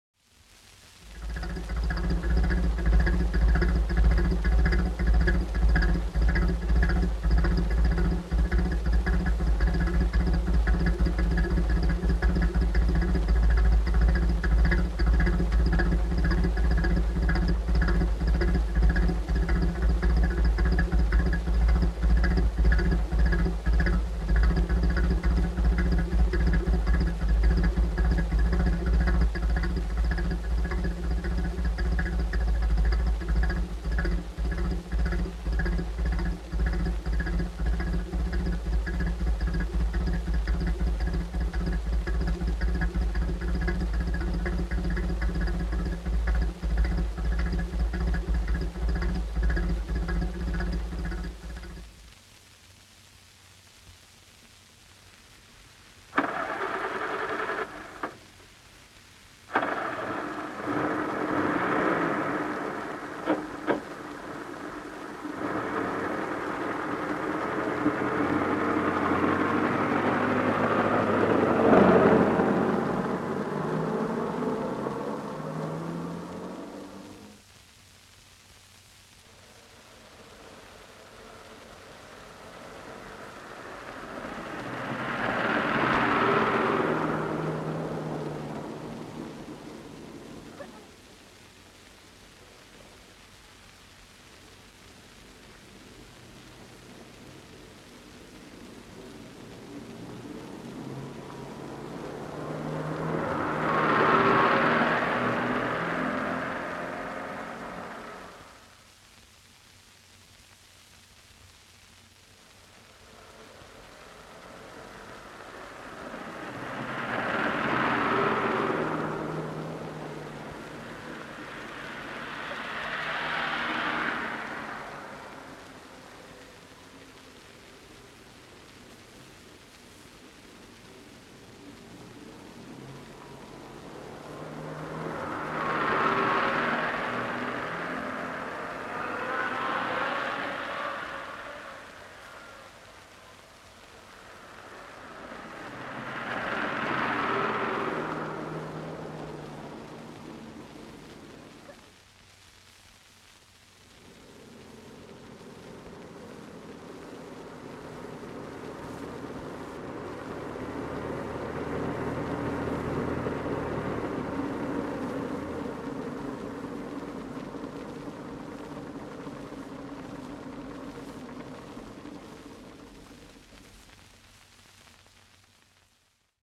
Archival Motor Car Sounds On HMV Part 2 (6K Subscribers Special)
Sound Effects: Motor Car Noises (1) Engine Running (2) Starting and Receding (3) Approaching and receding (4) Approaching and receding (5) Approaching and Stopping.